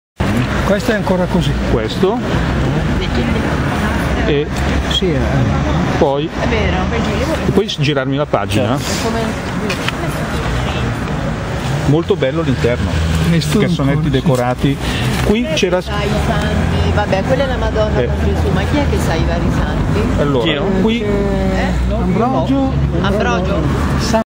Villa Fisogni - le foto degli interni
Villa Fisogni  in via Mazzini a Palazzolo Milanese (frazione di Paderno Dugnano) durante la Videopasseggiata dei Parchi